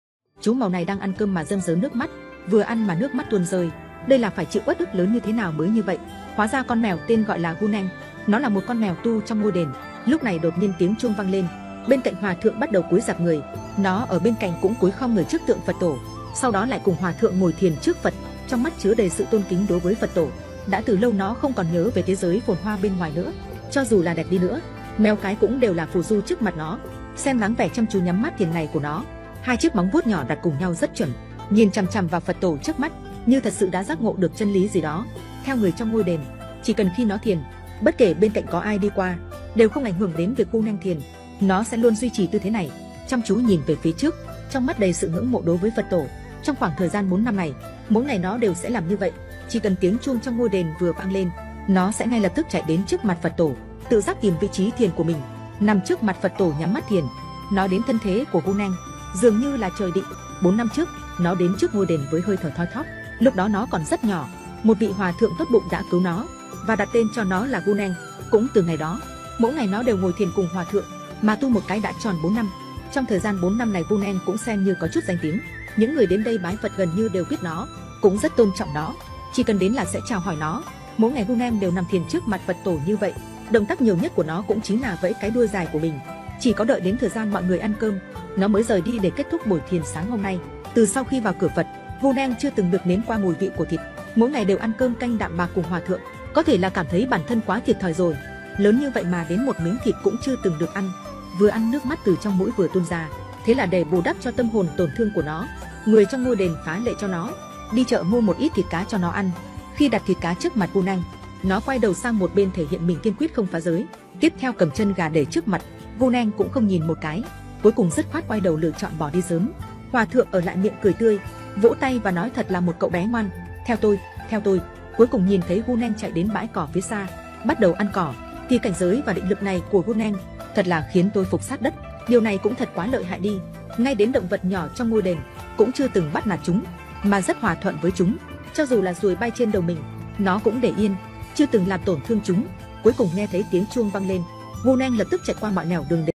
Mèo Mập Review – Con Mèo Quy Y Tam Bảo II A DI ĐÀ PHẬT
- Theo quan niệm của Đạo phật Lục đạo luân hồi là sáu nẻo luân hồi trong tam giới (dục giới, sắc giới, vô sắc giới).
- Do nghiệp lực của chúng sinh tạo ra, sẽ dẫn dắt chúng sinh đi vào các nẻo của lục đạo là : Địa ngục, Ngạ quỷ, Súc sinh, Cõi Người, Cõi A Tu La, Cõi Thiên.